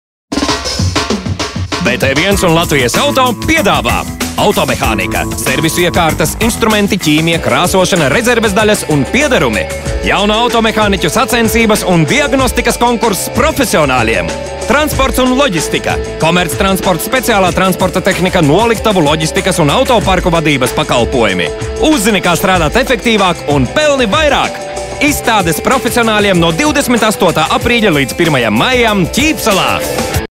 "Transports un loģistika" un "Automehānika" - TV klips - (28.04.-1.05.2011)
Biznesa izstāde "Transports un Loģistika" ir komplekss pasākums transporta un tranzīta nozares uzņēmējiem, vadošajiem ekspertiem un valsts amatpersonām, kura mērķis ir veicināt Latvijas transporta, tranzīta un loģistikas nozares un nozarē strādājošo uzņēmumu turpmāko attīstību.

No 28. aprīļa līdz 1. maijam Starptautiskajā izstāžu centrā Ķīpsalā notiks specializētā automobiļu remonta instrumentu, servisa iekārtu, rezerves daļu, apkopes līdzekļu un aksesuāru izstāde "Automehānika 2011", kas īpaši paredzēta autoservisu īpašniekiem un darbiniekiem, autoinženieriem, automehāniķiem, autonozares speciālistiem, kā arī ikvienam, kas pats remontē savu automobili un vēlas iepazīties ar nozares jaunumiem.